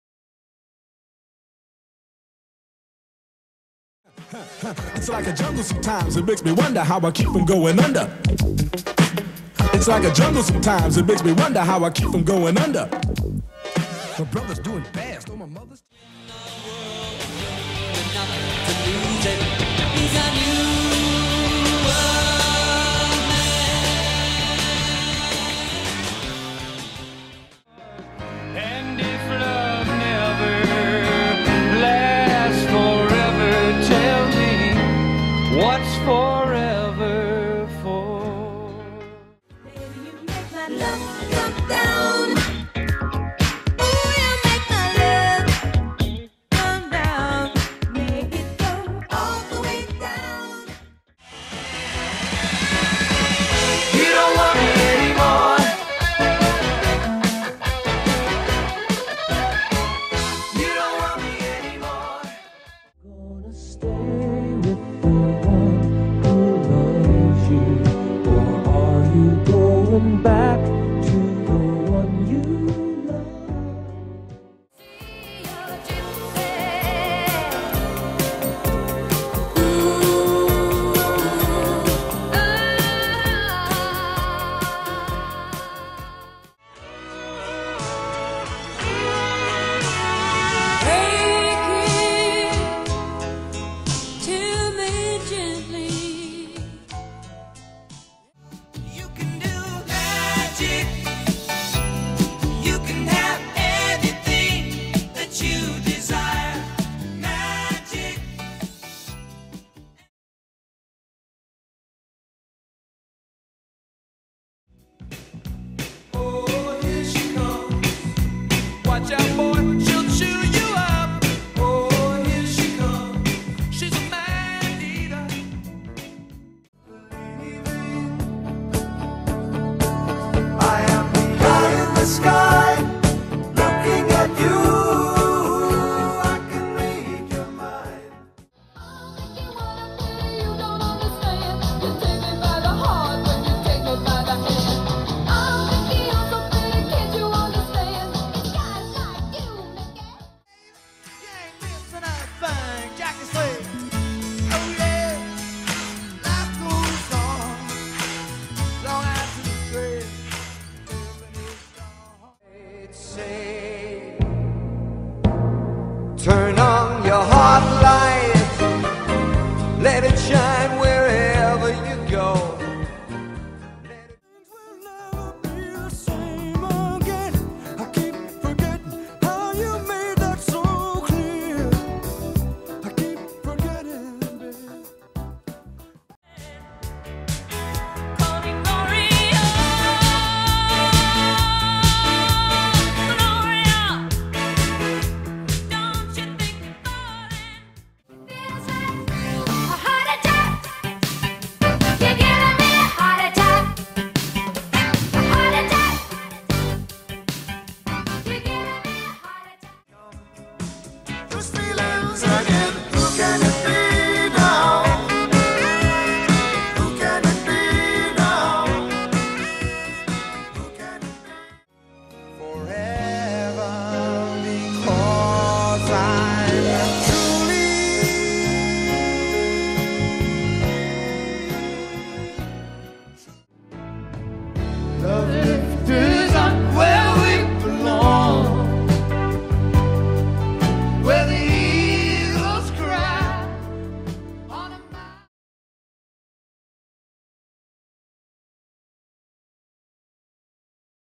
Top 10 US Songs NOV 82-Hall&Oates, Cocker&Warnes, Lionel Ritchie, Laura Branigan, Toni Basil, Neil D
334th video in a series of videos ranking the Top 10 songs for each month. The series is titled "Each Month's Top 10+ US Songs (1980s)". This video ranks the songs by their peak position within the given month (NOVEMBER 1982).